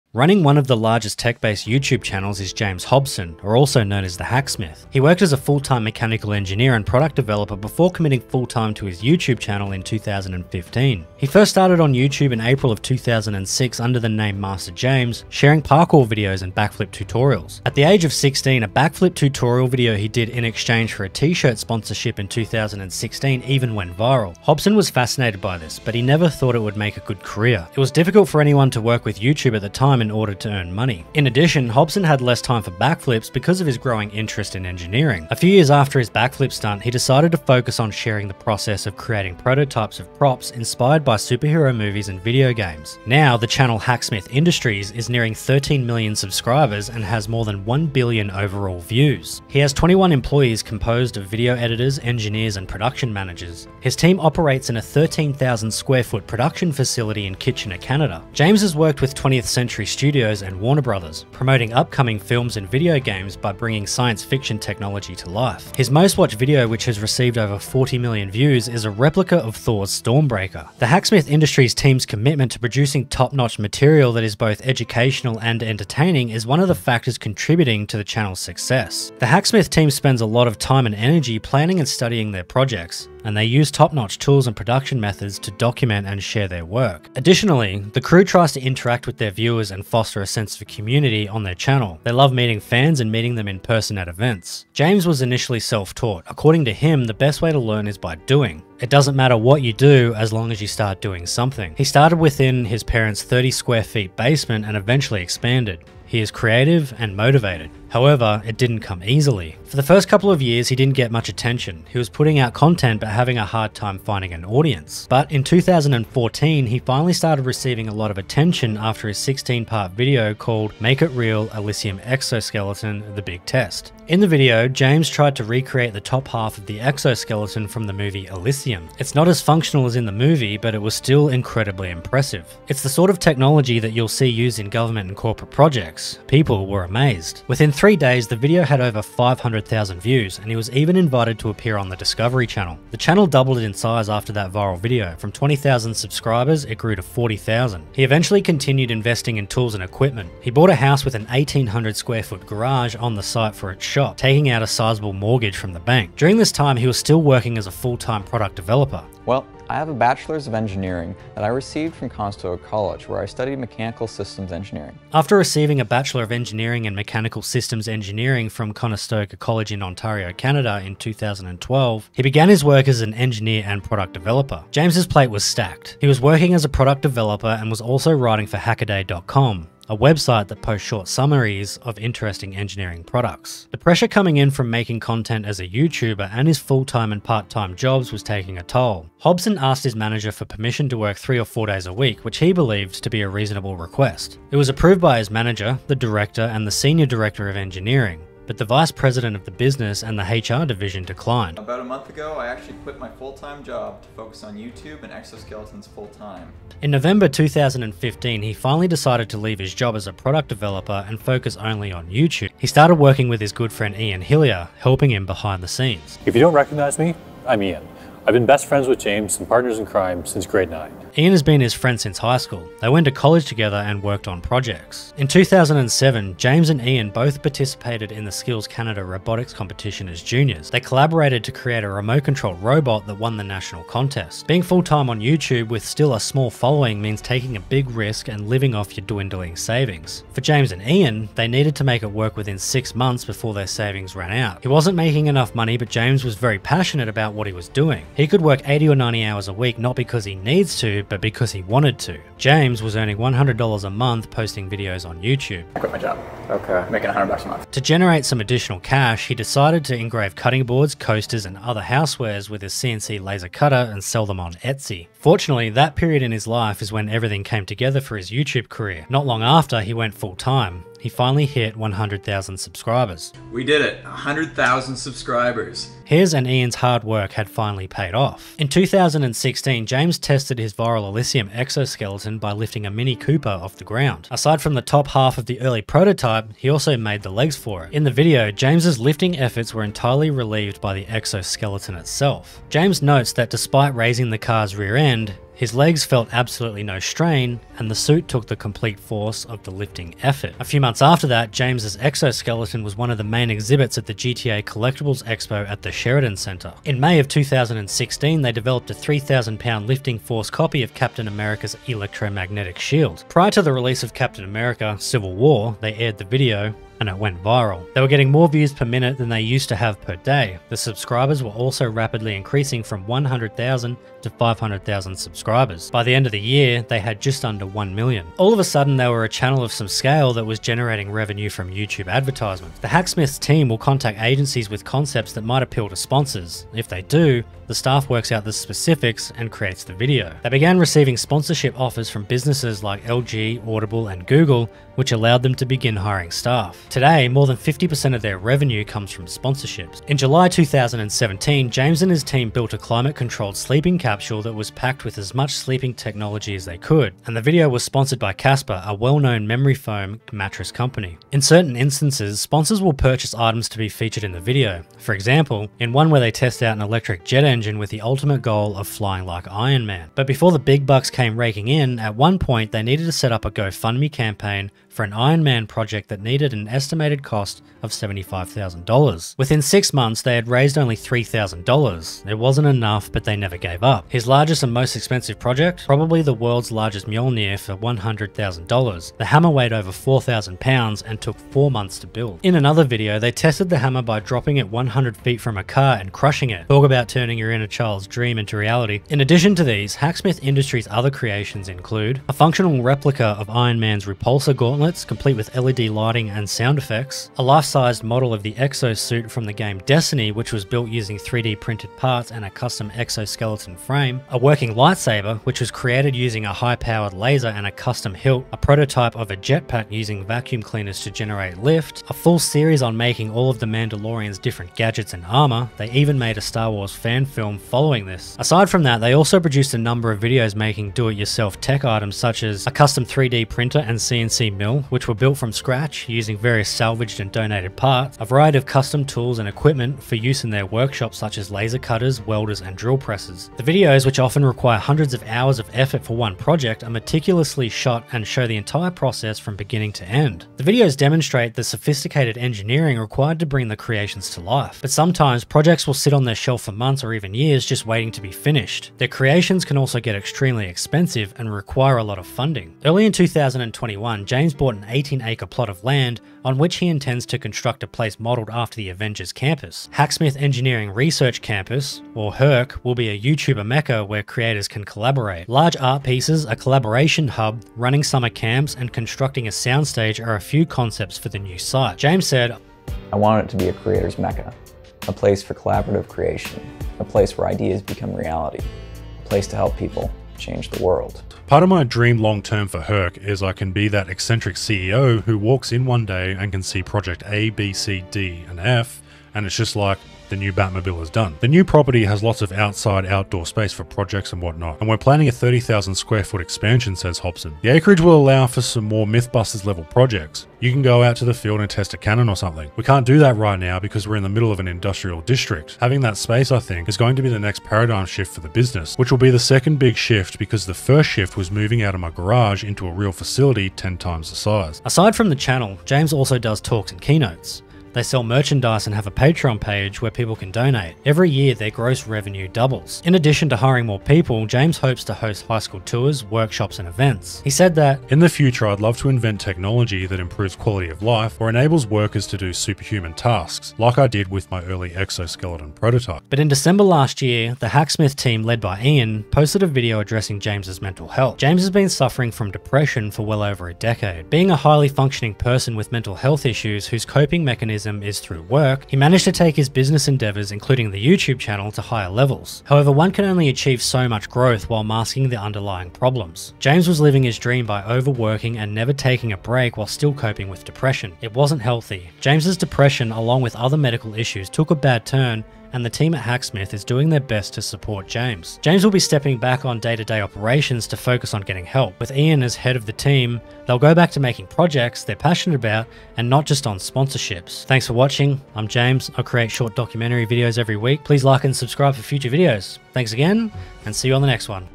Real Life Tony Stark | The Hacksmith Industries Story
I Stream Live Most Days

This is all about James AKA The Hacksmith from Hacksmith Industries! Some would say he is the real-life Tony stark! He is bringing many of our childhood dreams to life with his amazing inventions and making it real!

hacksmith industries
The Hacksmith
Make it real